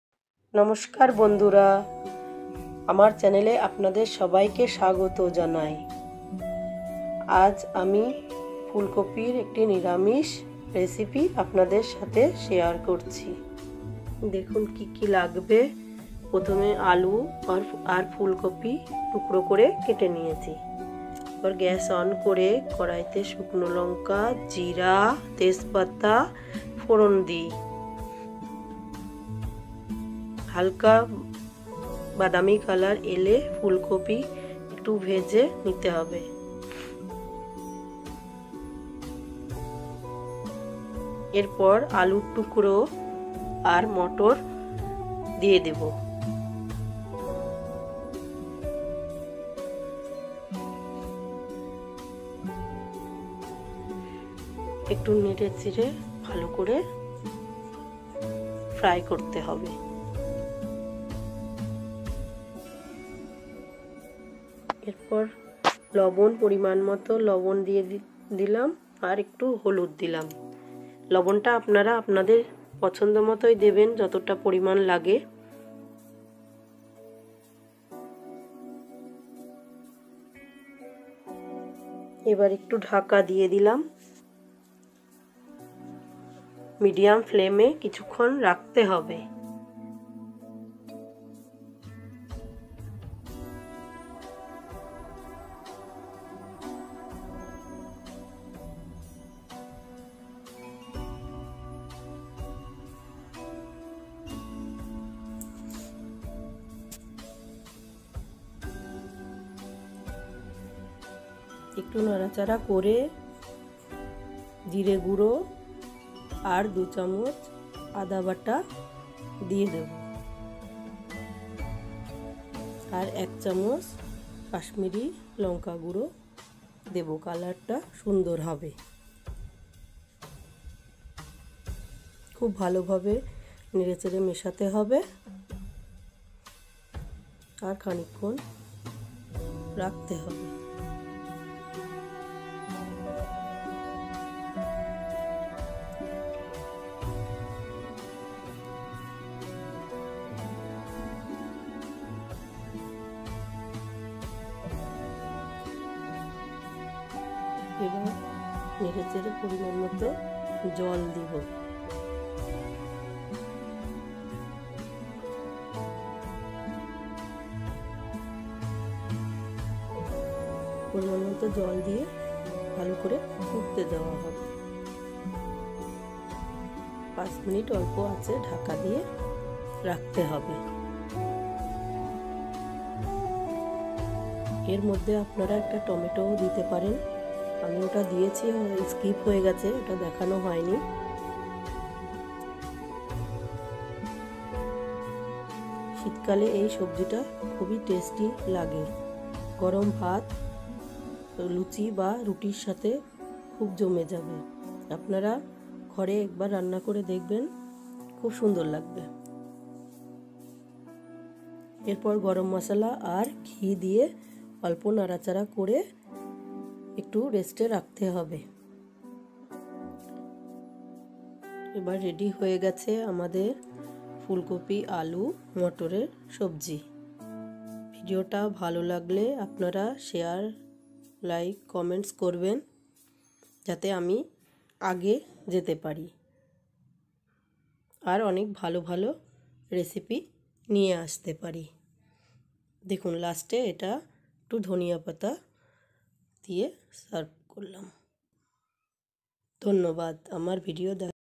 ফুলকপি,আলু,মটর এর নিরামিষ তরকারি ।গরম ভাত,রুটি ও লুচির সাথে খুব ভাল লাগবে।cauliflowerrecipe
নিরামিষ ফুলকপির তরকারি
How to make califlower